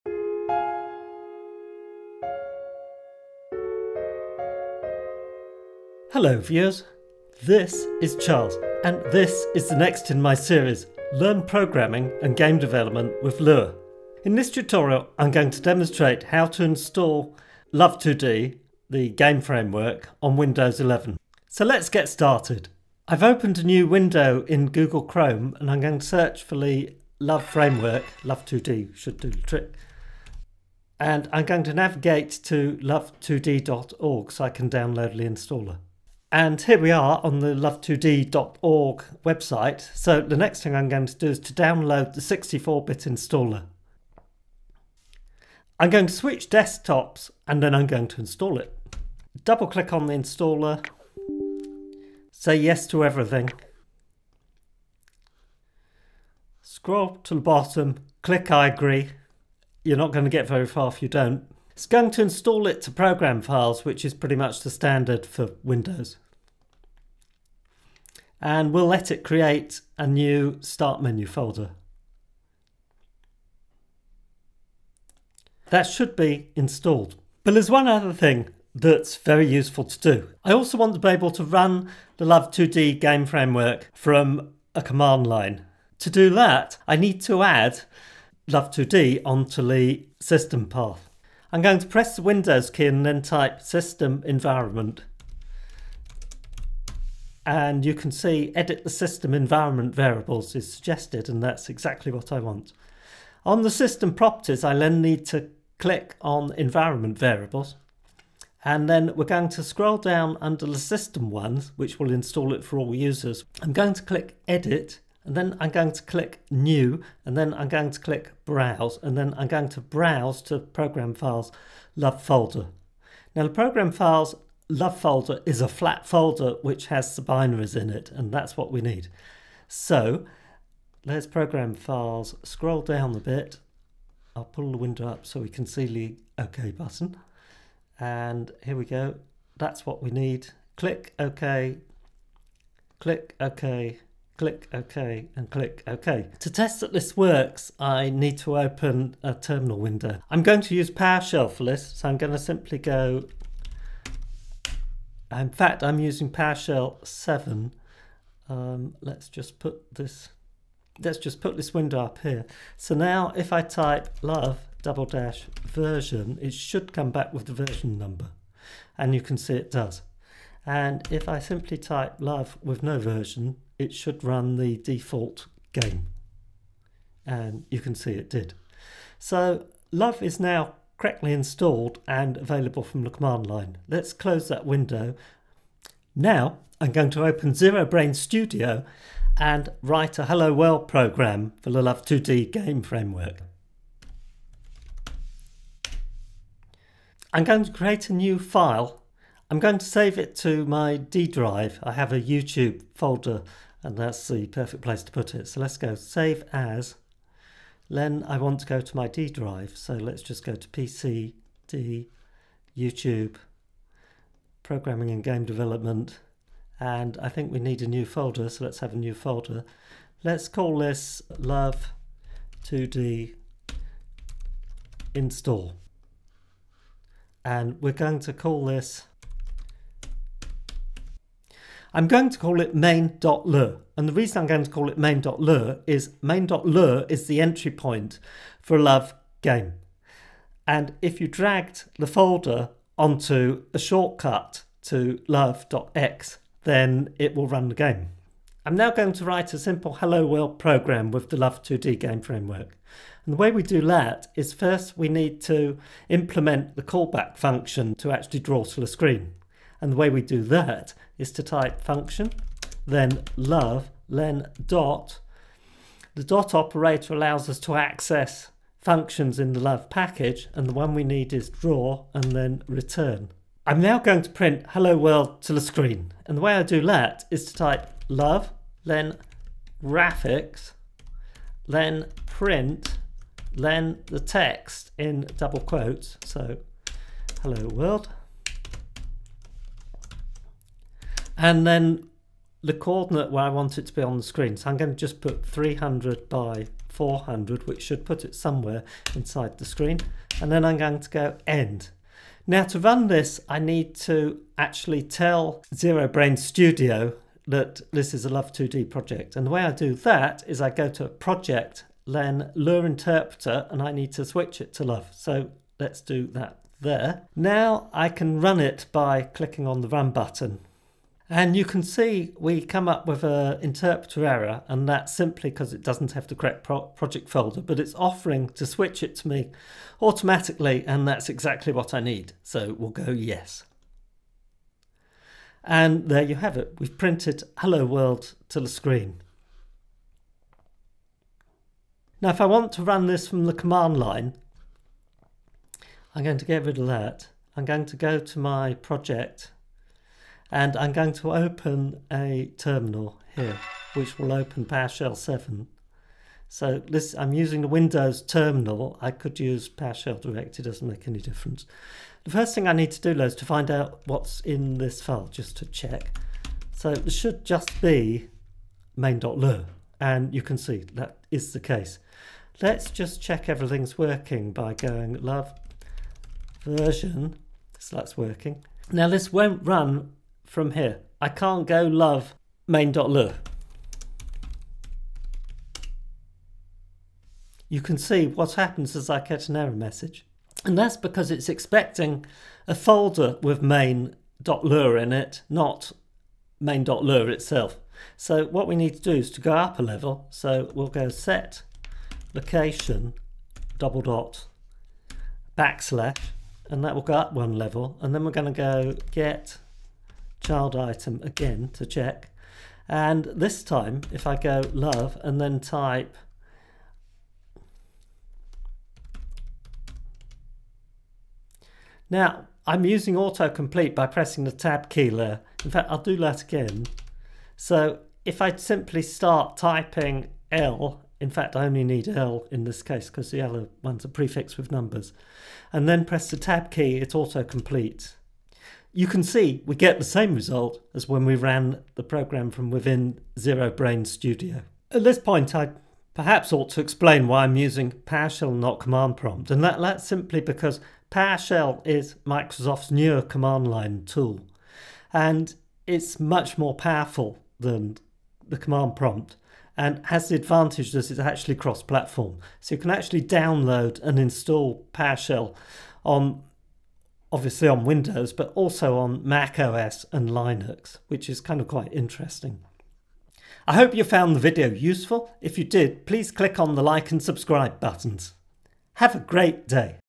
Install Love2D Windows
In this video I demonstrate how to install the LÖVE game framework, love2d, on Windows 11.
I show how to put the LÖVE game framework, love2d,  in the Windows System Path to allow love2d to be run from the command line.
Then I write a LÖVE, love2d, Hello World program to test the installation.

00:00 - Start
00:06 - Introduction
00:22 - Getting the Windows 11 Installer
00:49 - Windows 11 Installation
01:17 - Adding LÖVE, love2d, to the System Path
03:05 - Testing LÖVE, love2d, In ZeroBrane Studio
05:42 - Running LÖVE, love2d, from the Windows Command Line